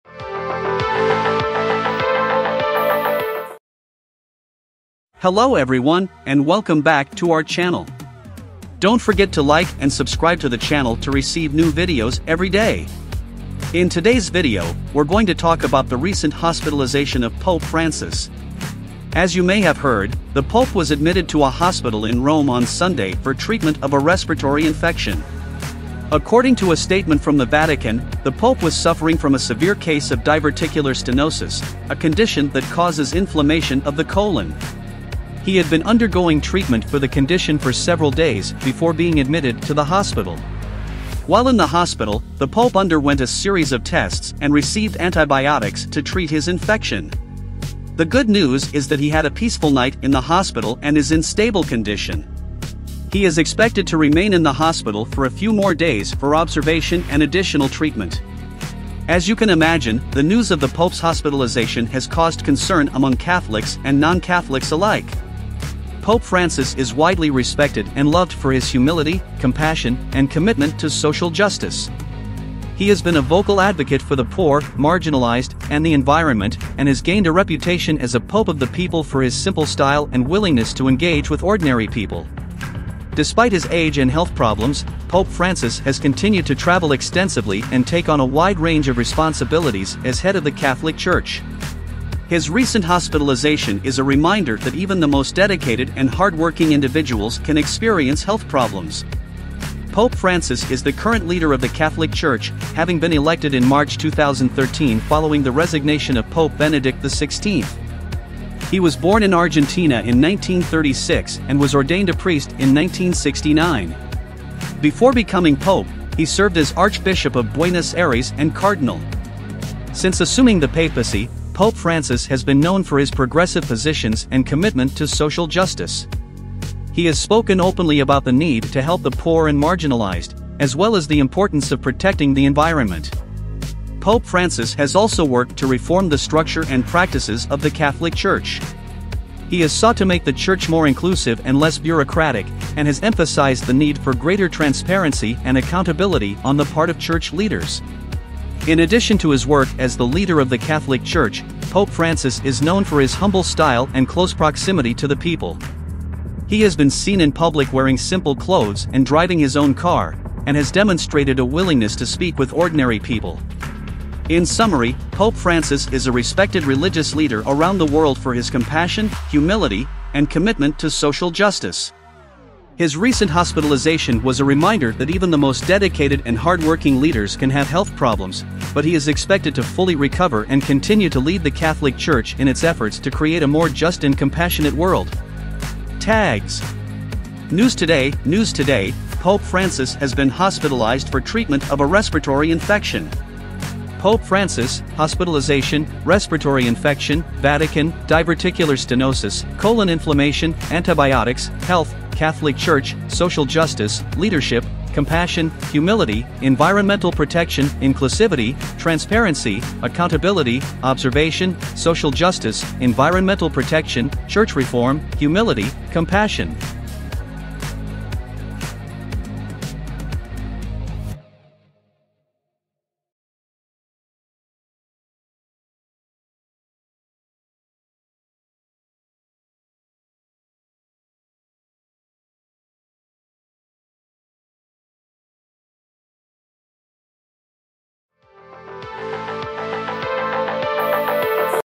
Pope Francis has been hospitalized for treatment of a respiratory infection
Join us as we discuss the recent hospitalization of Pope Francis, the leader of the Catholic Church.

Learn about his condition and the treatment he received, as well as the impact of his leadership on social justice and the environment.

Discover why he is widely respected and loved by Catholics and non-Catholics alike, and how his recent health scare serves as a reminder of the importance of self-care for even the most dedicated individuals.

 Don't miss out on this insightful discussion about one of the most influential religious figures in the world.